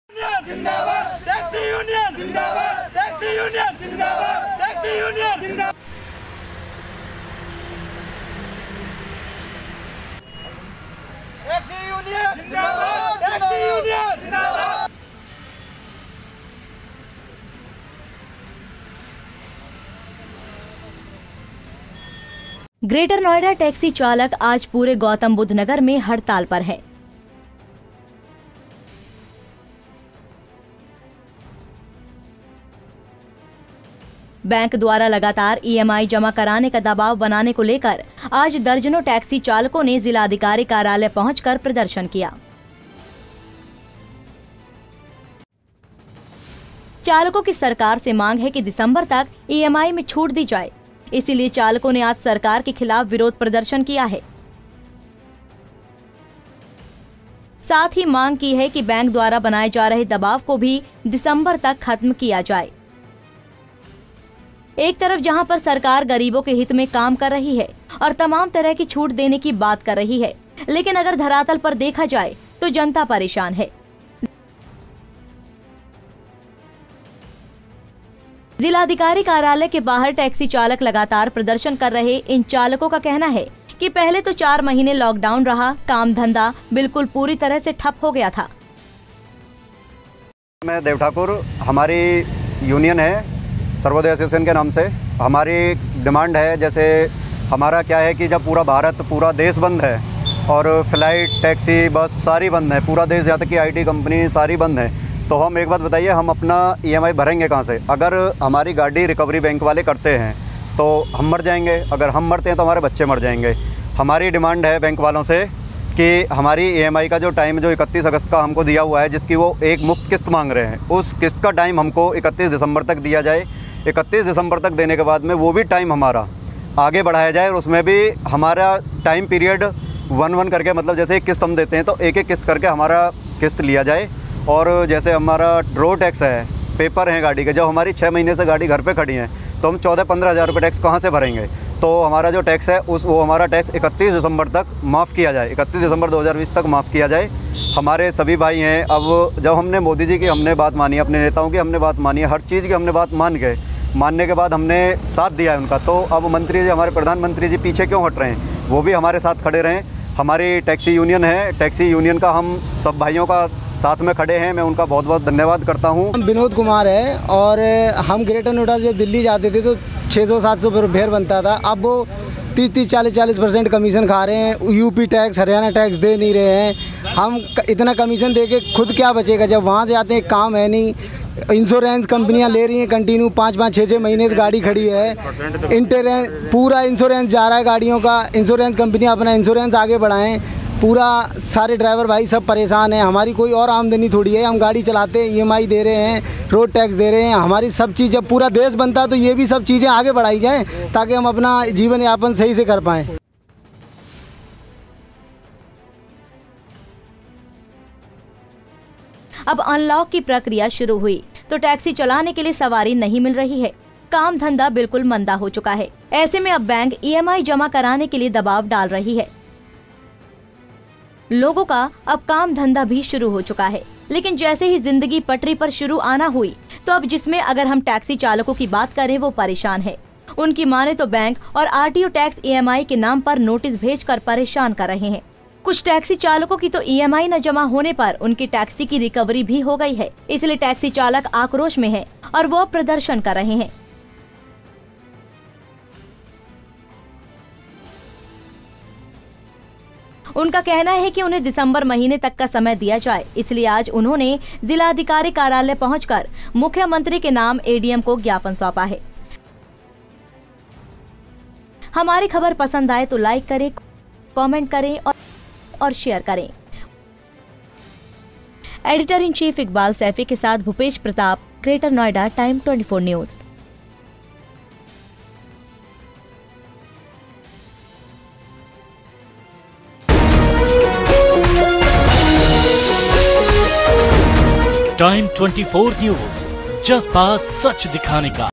टेक्सी चालकों की हड़ताल की ज़िम्मेदार सरकार या फाइनेंस कंपनी?
ग्रेटर नोएडा टैक्सी चालक आज पूरे गौतम बुध नगर में हड़ताल पर हैं, बैंक द्वारा लगातार EMI  जमा कराने का दबाव बनाने को लेकर आज दर्जनों टैक्सी चालकों ने जिलाधिकारी कार्यालय पहुंचकर प्रदर्शन किया।, चालकों की सरकार से मांग है कि  दिसंबर तक  EMI में  छूट दी जाए। इसीलिए चालकों ने आज सरकार के खिलाफ विरोध प्रदर्शन किया है। साथ ही मांग की है बैंक द्वारा बनाये जा रहे दवाब को भी दिसम्बर तक खत्म किया जाए।एक तरफ जहां पर सरकार गरीबों के हित में काम कर रही है और तमाम तरह की छूट देने की बात कर रही है, लेकिन अगर धरातल पर देखा जाए तो जनता परेशान है, जिलाधिकारी कार्यालय के बाहर टैक्सी चालक लगाकर प्रदर्शन कर रहे इन चालकों का कहना है कि पहले तो 4 महीने लॉकडाउन रहा काम धंधा बिल्कुल पूरी  तरह से ठप हो गया था, अब  अनलॉक  की प्रक्रिया शुरू हुई तो टैक्सी चलाने के लिए सवारी नहीं मिल रही है काम धंधा बिल्कुल मंदा हो चुका है ऐसे में अब बैंक EMI जमा कराने के लिए दबाव डाल रही है।लोगों का अब काम धंधा भी शुरू हो चुका है लेकिन जैसे ही जिंदगी पटरी पर शुरू आना हुई तो अब जिसमें अगर हम टैक्सी चालकों की बात करें वह परेशान है उनकी माने तो बैंक और आरटीओ  टैक्स EMI के नाम पर नोटिस भेज कर परेशान कर रहें है, कुछ टैक्सी चालकों की तो एमआई ना जमा होने पर उनकी टैक्सी की रिकवरी भी हो गई है, इसीलिए टैक्सी चालक आक्रोश में है और वह अब प्रदर्शन कर रहे हैं उनका कहना है कि उन्हें दिसंबर महीने तक का समय दिया जाए इसलिए आज उन्होंने जिलाधिकारी कार्यालय पहुंचकर मुख्यमंत्री के नाम  एडीएम को ज्ञापन सौंपा है।